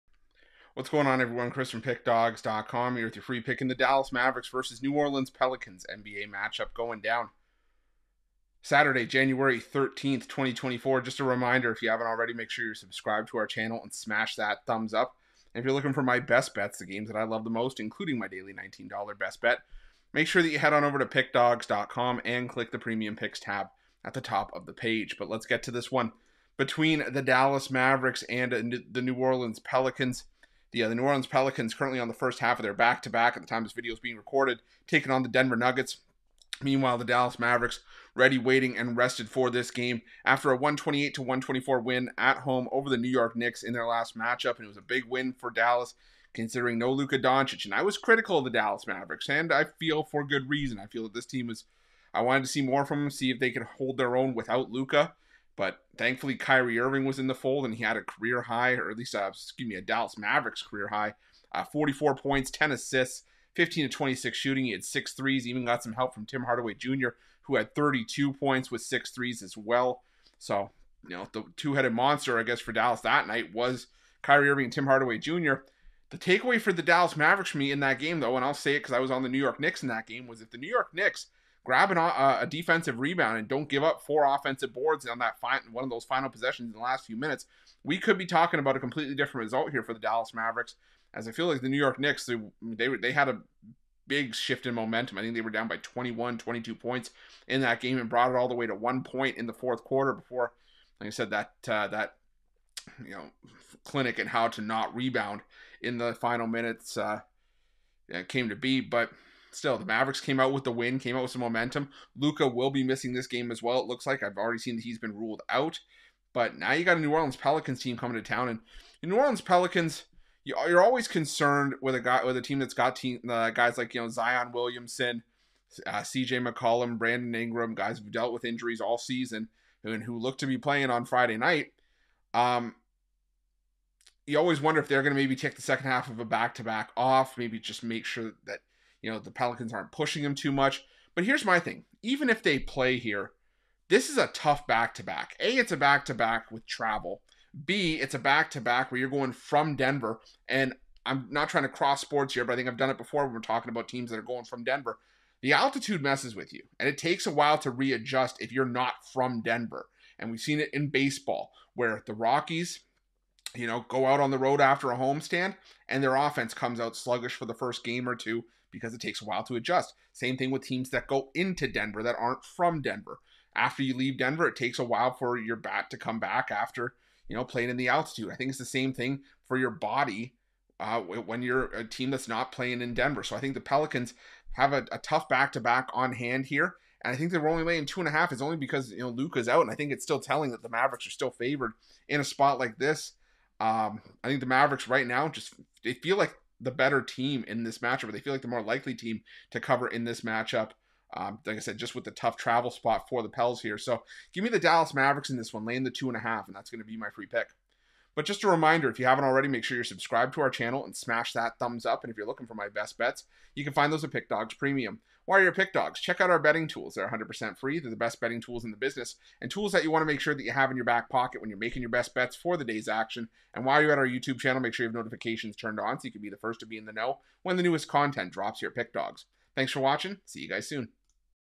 Dallas Mavericks vs New Orleans Pelicans 1/13/24 NBA Free Picks & Predictions | NBA Betting Tips
The New Orleans Pelicans face the Dallas Mavericks in Saturday's NBA Basketball action. Get Chris' NBA Pick and Prediction for this game.